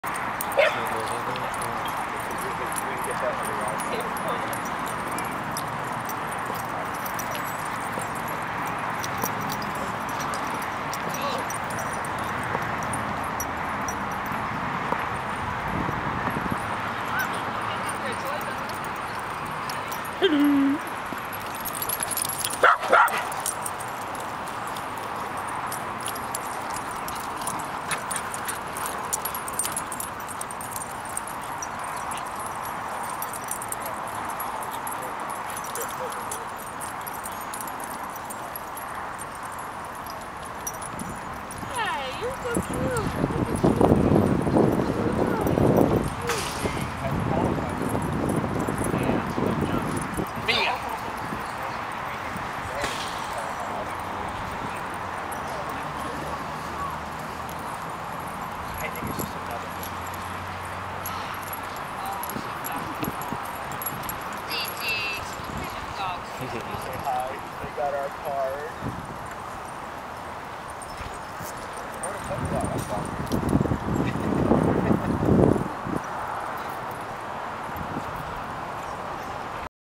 Leo, Stitches and Gigi English Bull Terriers at Dog Park #3
Video 3:  Leo, Stitches and Gigi English Bull Terriers at Los Gatos Creek Dog Park.  This was their first meeting on Mother;s Day, 5/8/2011.